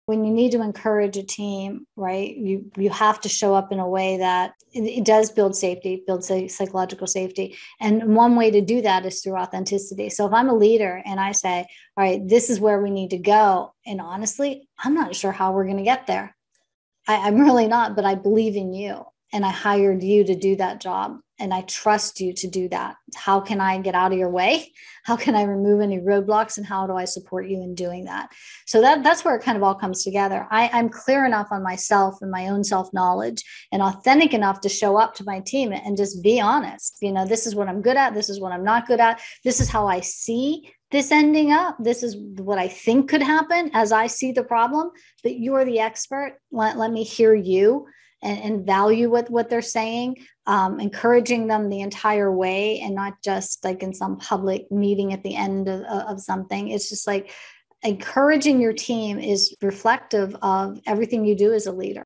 Encourage the Team
People have mixed feelings about exit interviews. On the one hand, an employee never wants to burn a bridge so you might not be getting the full truth on why someone is leaving.

On the other hand, sometimes you’ll find little patterns in the phrases and reasons given by multiple people. Recently I was on a call with a client and he told me that as people resigned from his company, several of them gave different variations of the same reason.

Some called it ‘not feeling heard,’ others said they were ‘afraid to speak up.’ One even said she is terrified of making a mistake. But no matter what they called it, it was clear that there was no psychological safety and it was affecting his business and impacting his bottom line.

In order to uncover what was really happening, my client and I dove in during our coaching call. What we discovered was that our teams need to see us take risks, they need to see that we are okay with mistakes, okay with failure. Our own, and theirs. Because that’s when they are going to be willing to take risks, be willing to speak up, and instead of resigning they’re willing to give us their best.

 Here’s a brief clip from that conversation..maybe it will help you too.